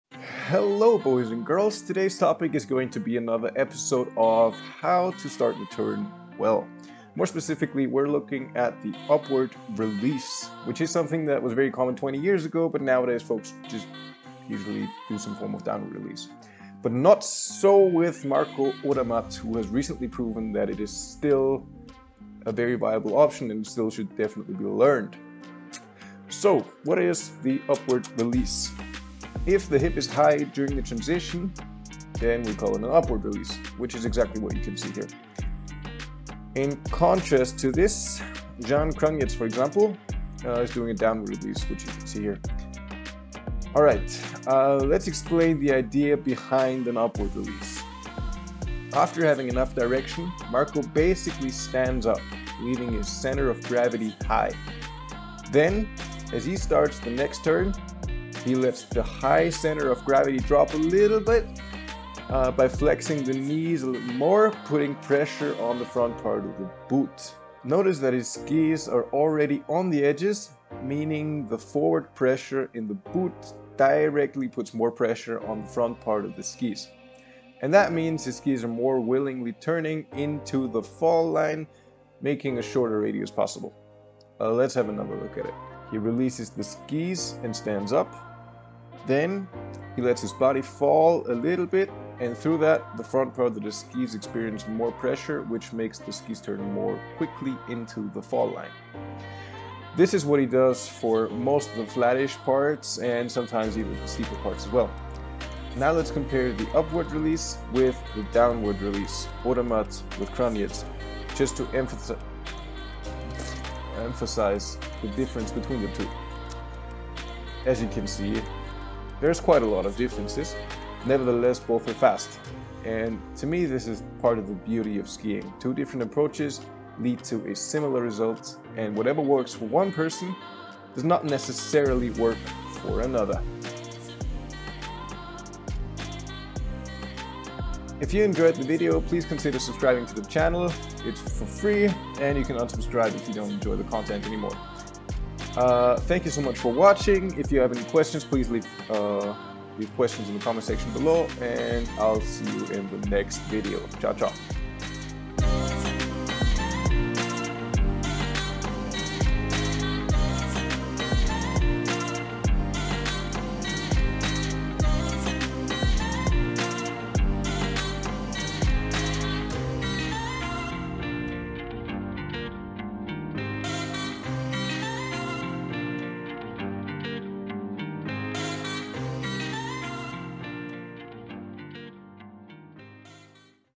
Ski Like Marco Odermatt (Upward Release)
Upward releasing is something few people do, but Marco Odermatt shows why  you should definitely learn how to do it!

Check out Marcos´s Instagram here: @marcoodermatt
Check out Zan´s Instagram here: @zankranjec

Do you want me to analyse your Video here on the channel?